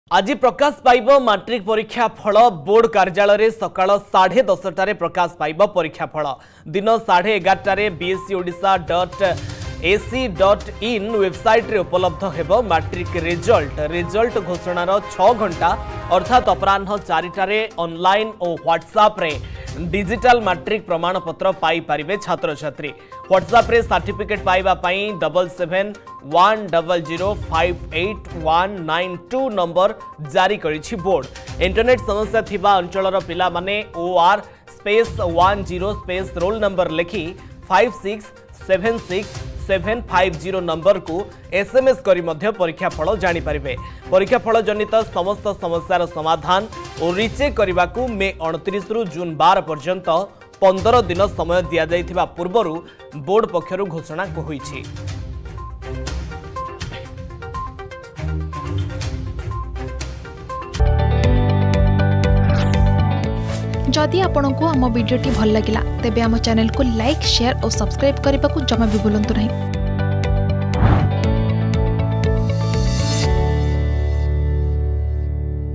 ଆଜି ପ୍ରକାଶ ପାଇବ ମାଟ୍ରିକ୍‌ ପରୀକ୍ଷାଫଳ || Matric Result
ଆଜି ପ୍ରକାଶ ପାଇବ ମାଟ୍ରିକ୍‌ ପରୀକ୍ଷାଫଳ ।  ବୋର୍ଡ କାର୍ଯ୍ୟାଳୟରେ ସକାଳ ସାଢେ 10ଟାରେ ପ୍ରକାଶ ପାଇବ ପରୀକ୍ଷା ଫଳ । ଦିନ ସାଢେ 11ଟାରେ bscodisha.ac. in ୱେବସାଇଟରେ ଉପଲବ୍ଧ ହେବ ମାଟ୍ରିକ ରେଜଲ୍ଟ ।